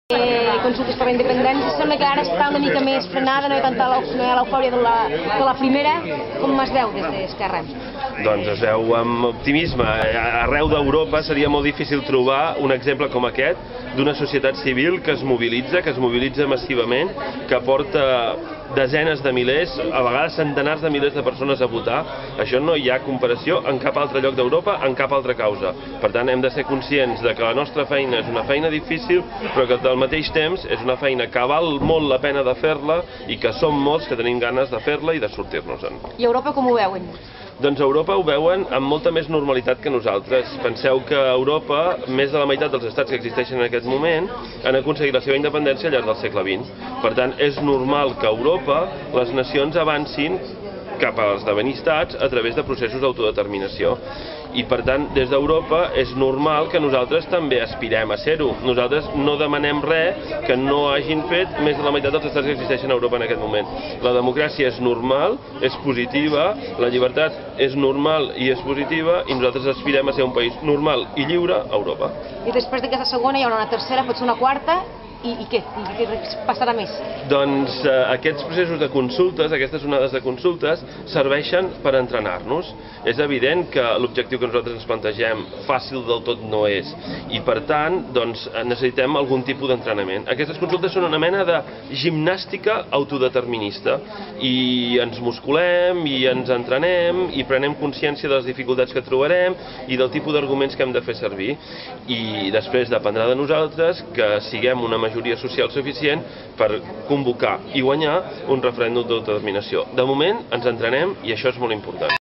Europa veu amb normalitat que Catalunya aspiri a la Independència per mitjà de la democràcia
degut a que més de la meitat dels estats dEuropa han aconseguit ser independents al llarg del segle XX, la onada de referèndums a Catalunya és una activitat vista amb normalitat.